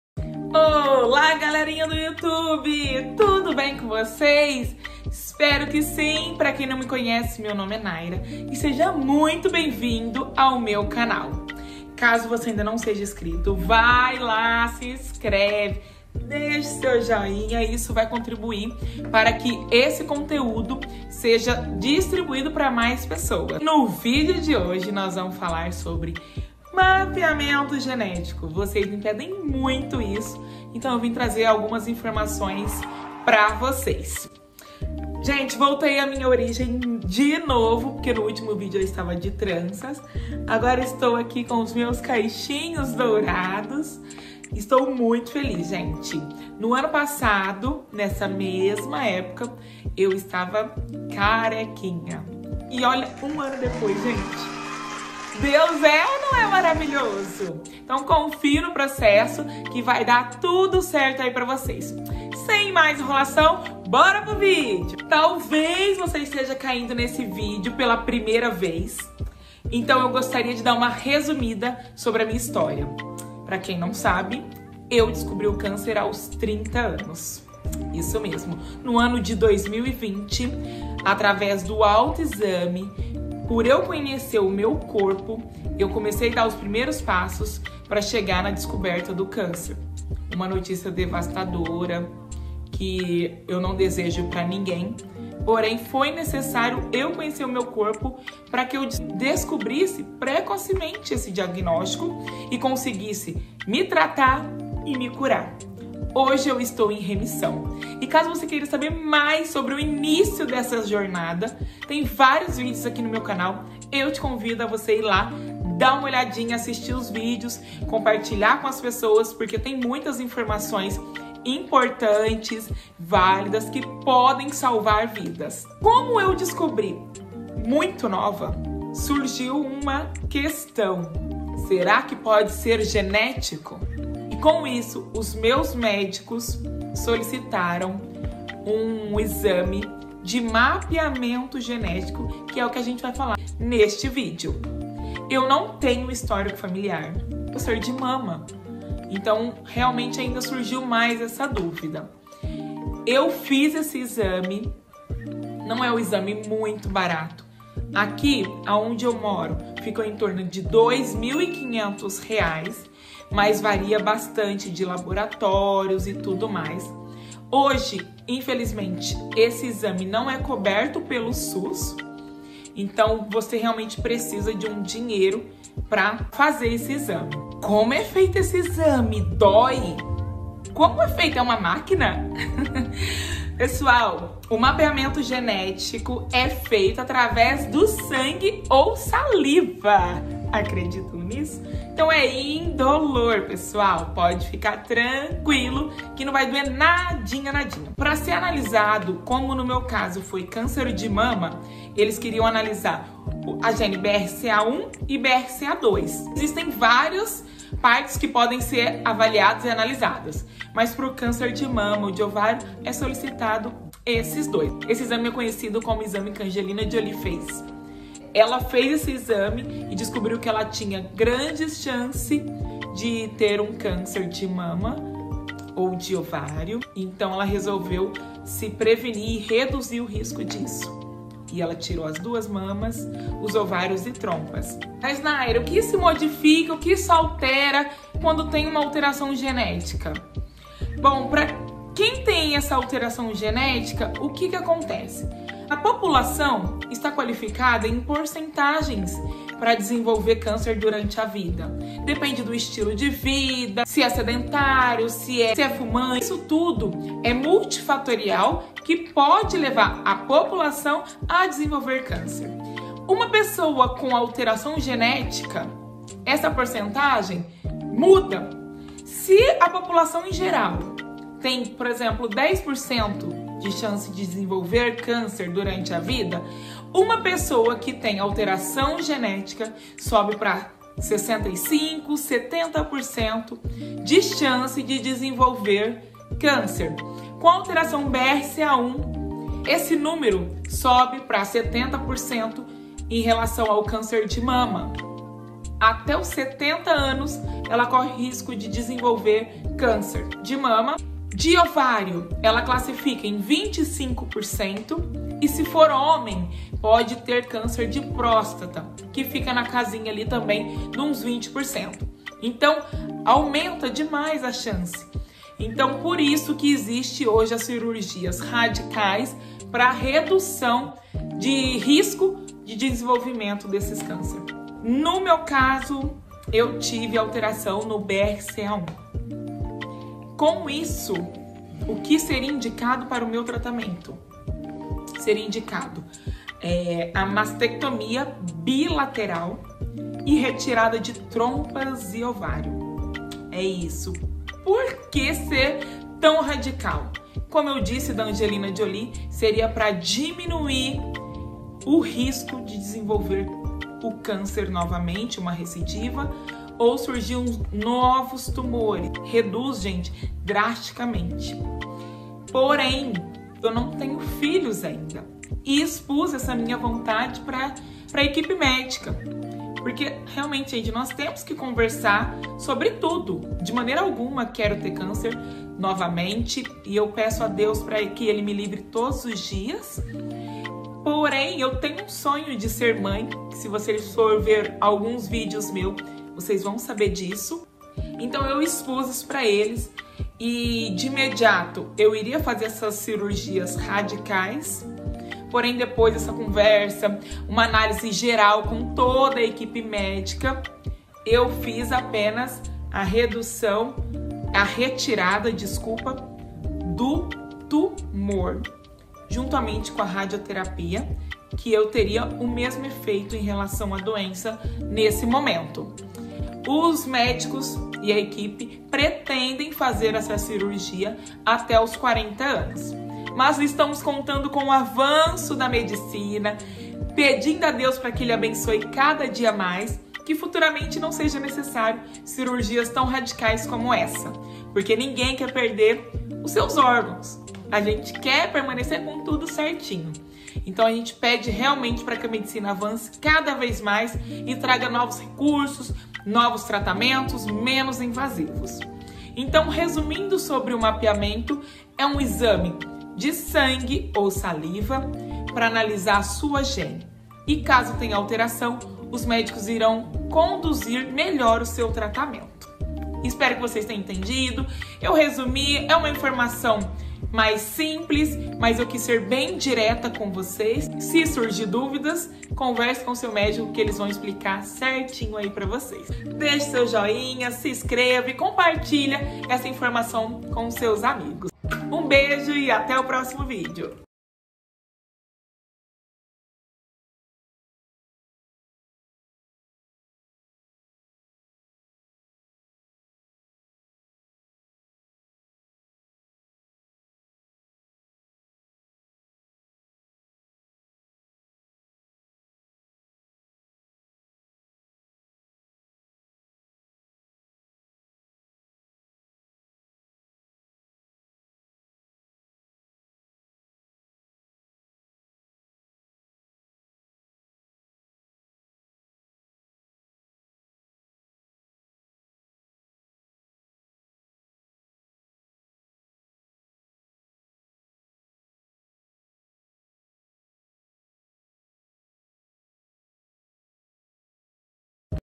✨Câncer genético ? Como descobri✨
Olá galerinha nesse vídeo eu conto sobre mapeamento genético, se ficar alguma dúvida pode me chamar 🧡